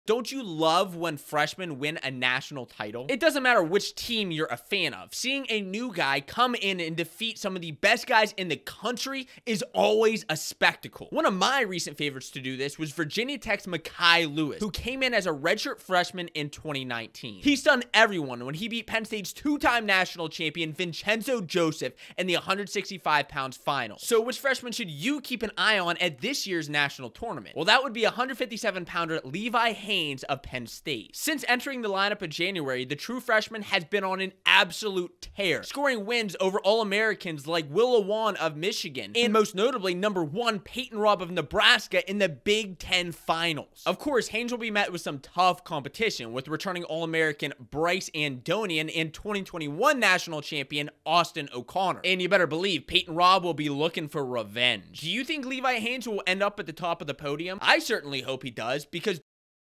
This Freshman Could WIN Nationals!
We all LOVE seeing freshmen win National titles. Seriously, there's just something special about it. But can a young guy win an NCAA Wrestling Championship in 2023? Here's the lowdown...
*CREDITS*
Script: Steve Guzzo
Editor: Josiah Hritsko